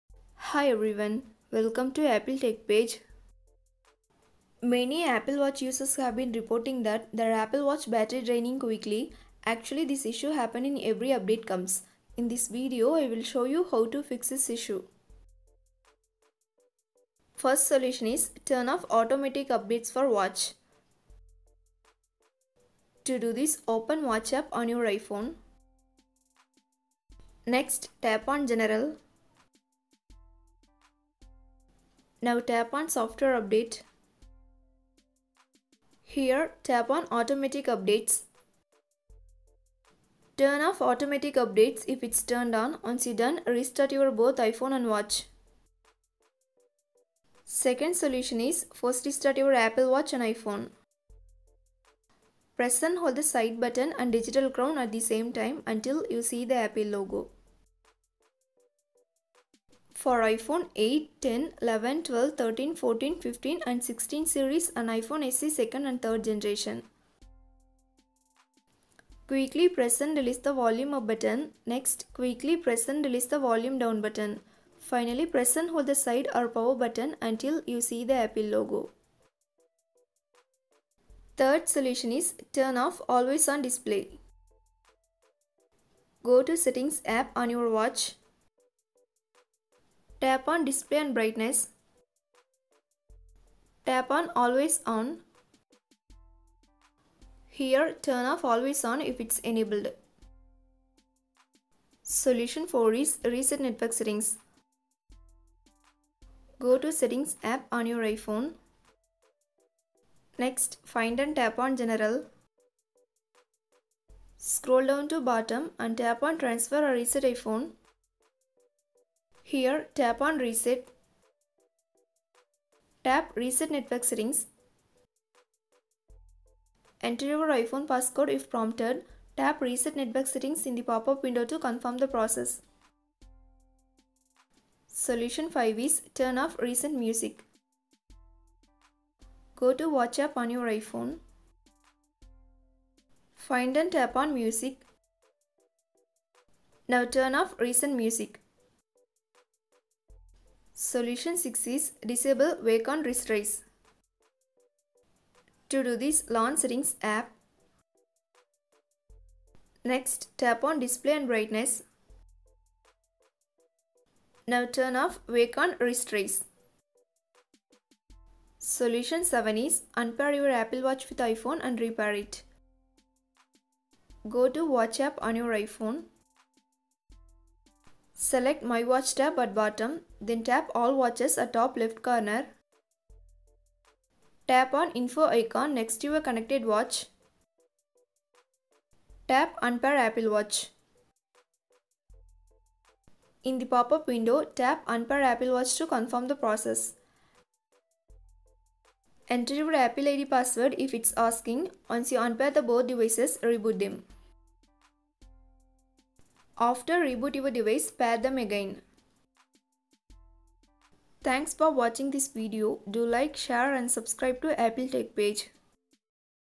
How to Fix Apple Watch Battery Draining Quickly after watchOS 11?
watchOS 11 battery draining quickly, Apple watch battery draining fast after watchOS 11, Apple watch SE battery draining after watchOS 11.